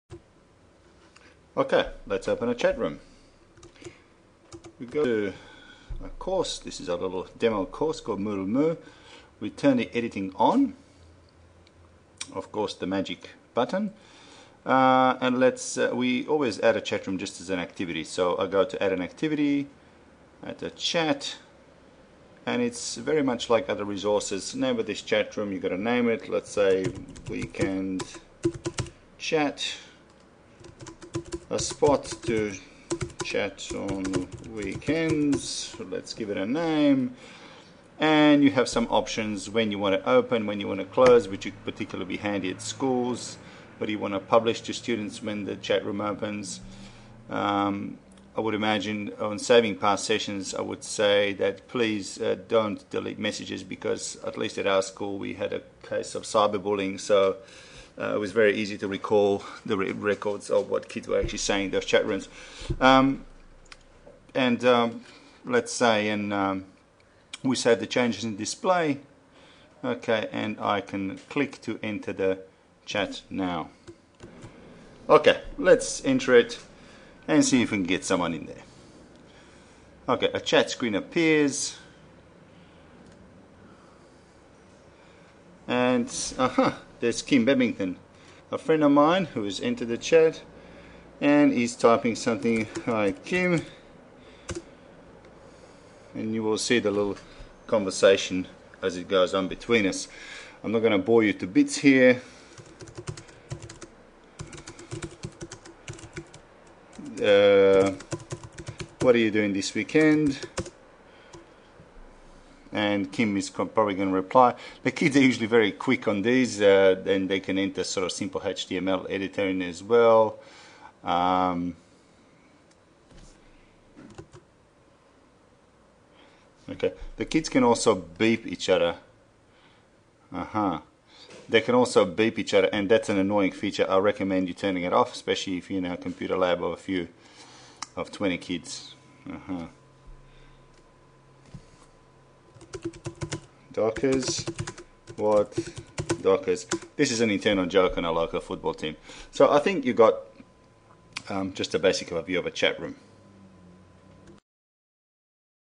Opening a chatroom in Moodle
A clip on opening and participating in a Moodle chatroom in Moodle 1.9. Please note that you need to have appropriate editing rights assigned by your Moodle admin to do so.